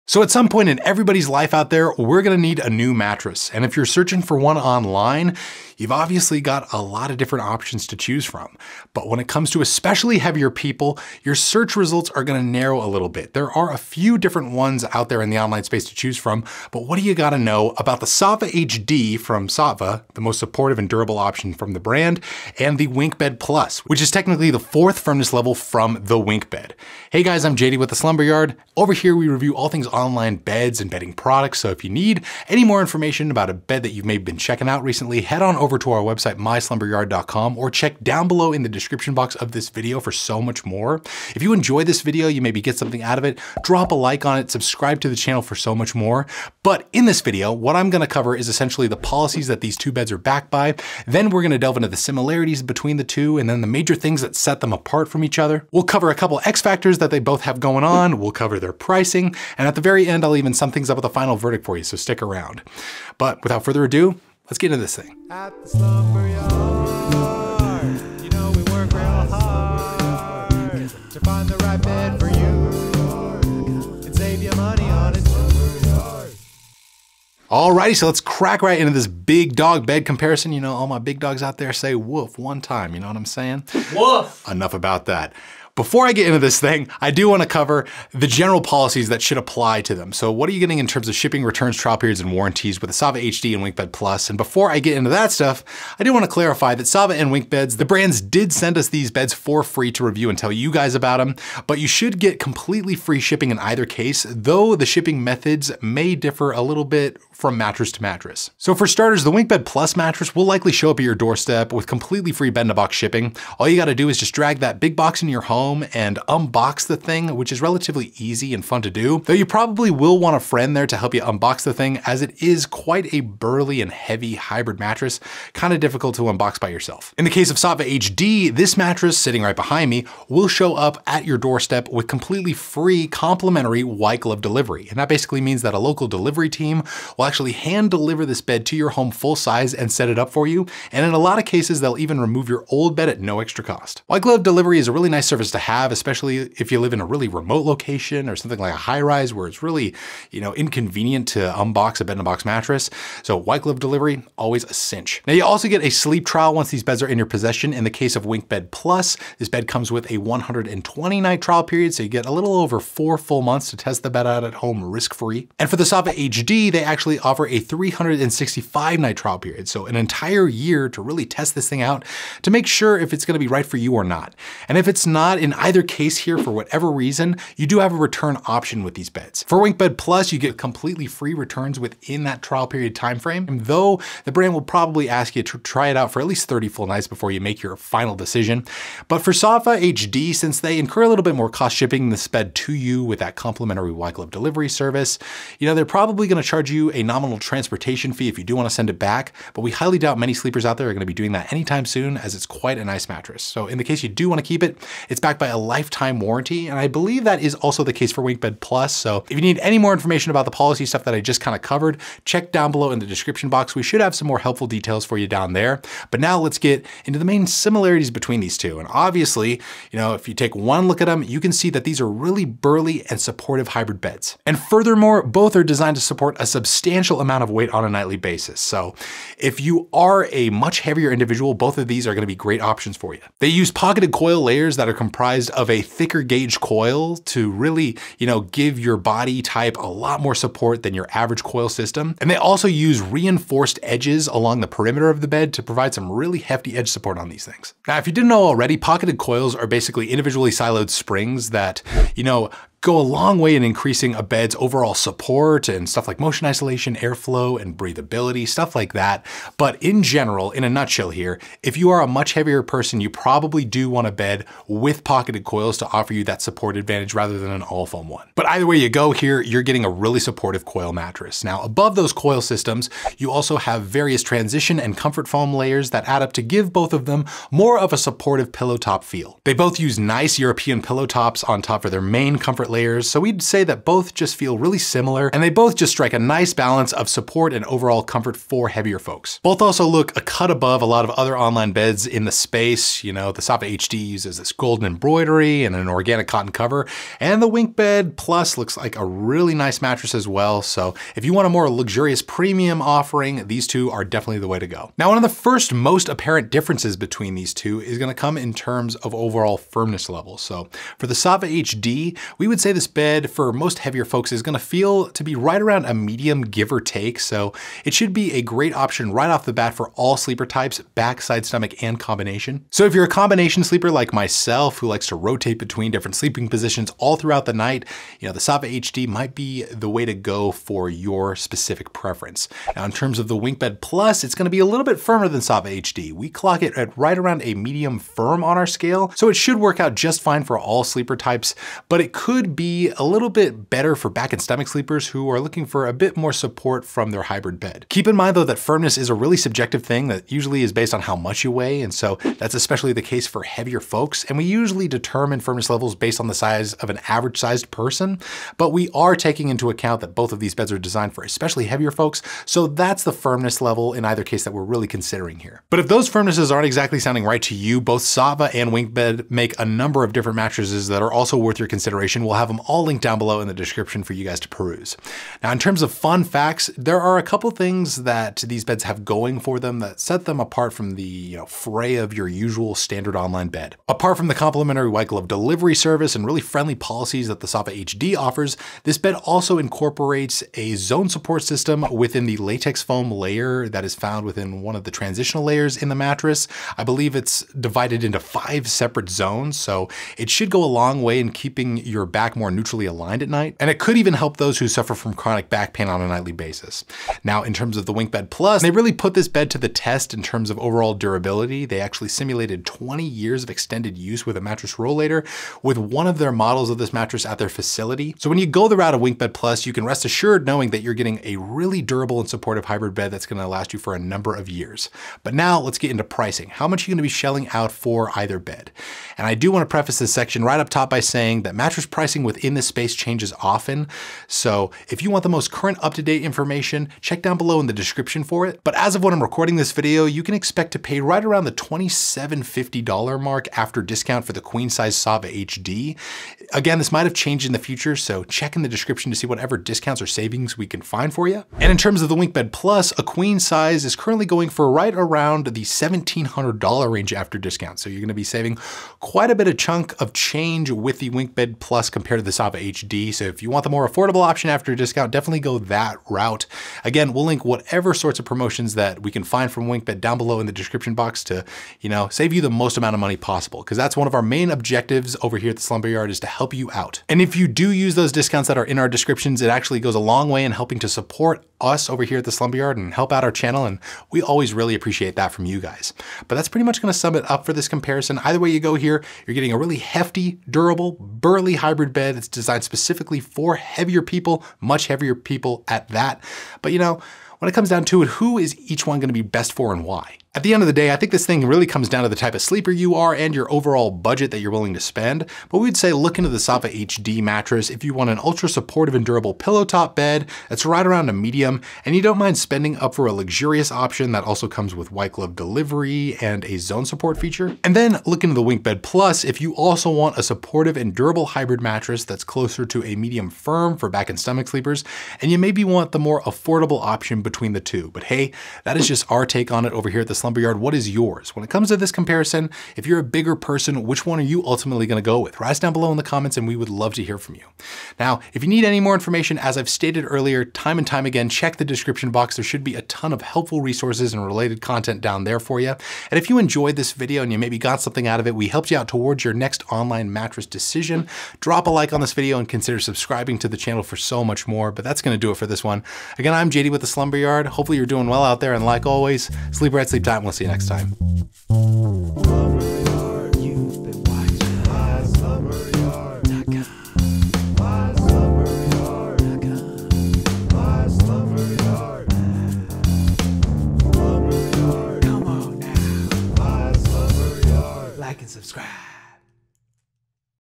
Saatva HD vs WinkBed Plus | Mattress Review For Heavy People
Click "more" For Advertiser Disclosure ↓↓
    (SALE: Up To $250 Off)

     (Sale: $300 Off + Free Sheets & Pillows)

Saatva HD vs WinkBed Plus | Mattress Review For Heavy People. In this video, JD who is a Certified Sleep Science Coach, covers our updated comparison of WinkBed Plus vs Saatva HD. These are two luxury hybrid mattresses you can buy online. Oftentimes, these two online mattresses are compared to premium hotel mattresses. JD covers the construction of each bed (innerspring unit, pocketed coils, memory foam, pillow top, etc) as well feel and firmness. The WinkBeds HD and Saatva HD mattresses are beds that are designed for heavier individuals. JD also discusses polices for these online beds including shipping, returns, trial periods and warranties. Of course, he also compares pricing and discounts for Saatva HD and WinkBed Plus as well. Be sure to see if these beds make our Best Mattress for Heavy People list for 2023.

fzgE

▬ VIDEO CHAPTERS ▬
0:00 - Introduction
1:19 - General Mattress Policies
3:36 - Similarities
5:05 - Differences
6:15 - X-Factors
7:05 - Mattress Pricing & Discounts
8:17 - Final Verdict: Saatva HD vs WinkBed Plus
8:49 - Conclusion